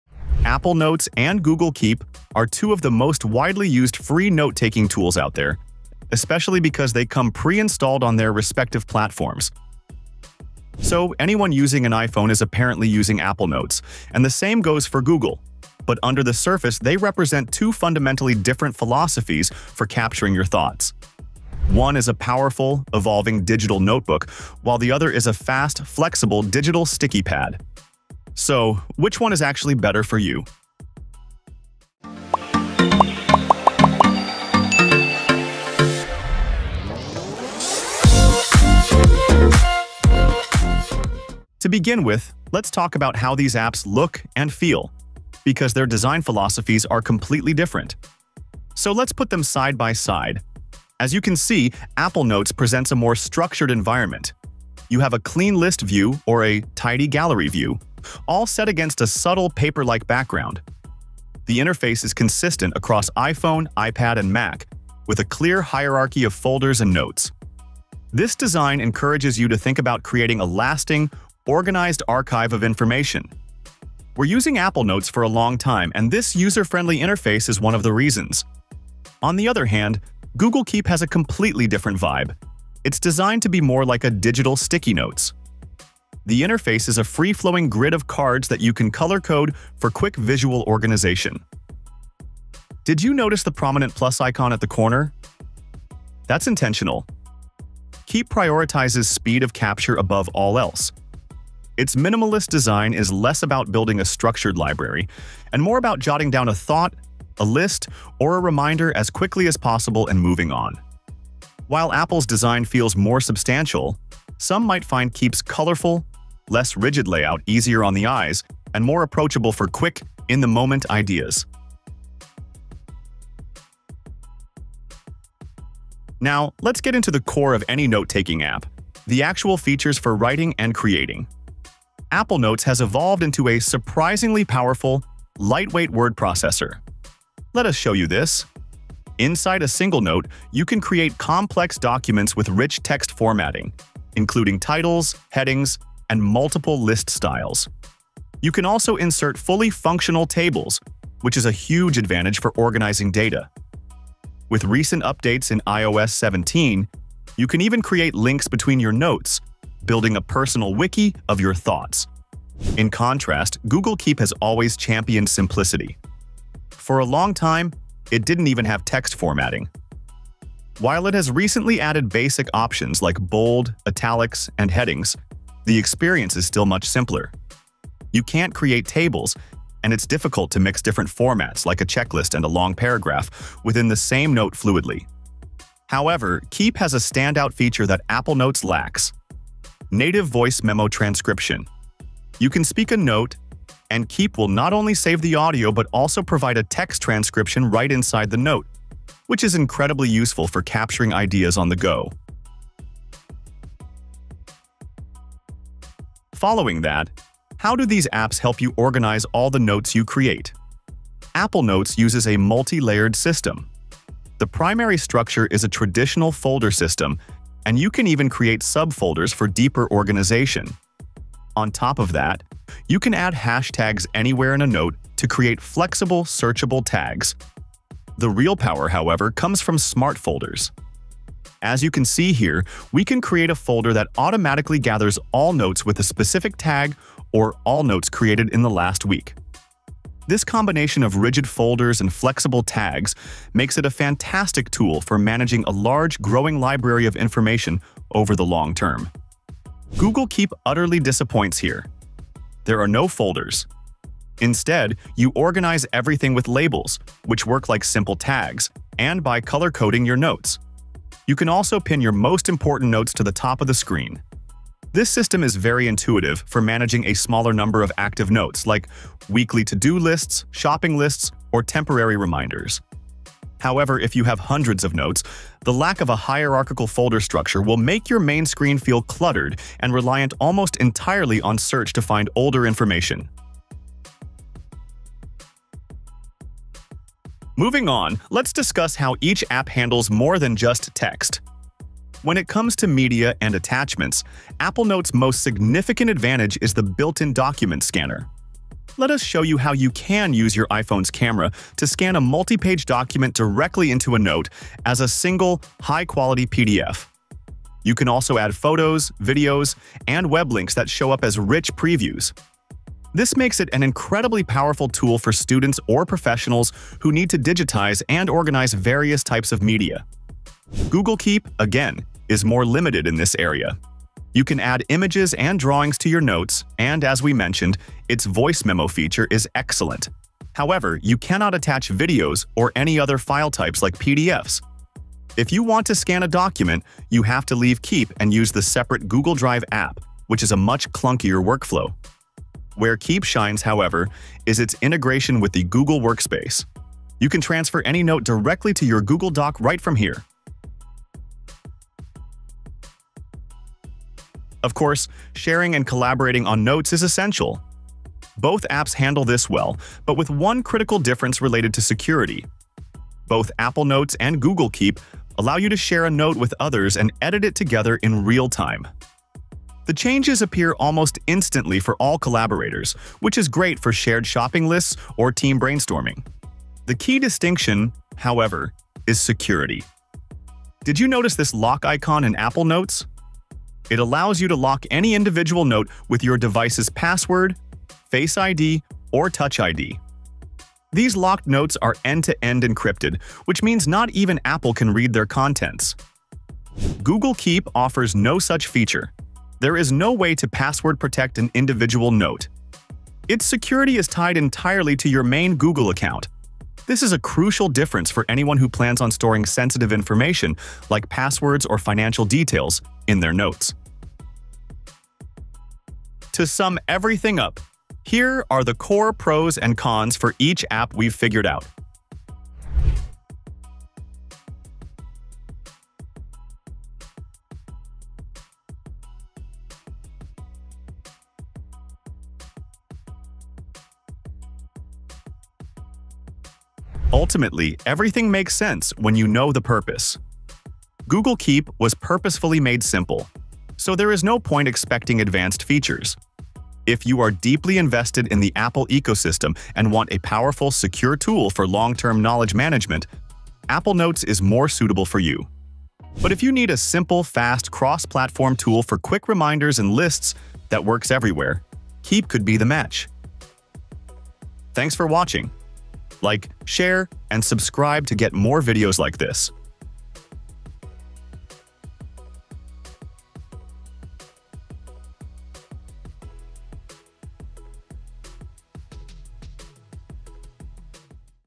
Apple Notes vs Google Keep | Which is Better for Notes Taking?
Apple Notes vs Google Keep, which free note-taking app is better? We dive deep into the latest features on iOS 17 and Android 14, comparing everything from interface and organization to collaboration and security. This is our definitive, in-depth review to help you choose the right app for your workflow.

Apple Notes vs Google Keep

00:00 Intro
00:39 Interface
02:03 Note-Taking Features
03:23 Organization
04:53 Media Integration & Attachments
05:52 Collaboration & Sharing
06:56 Outro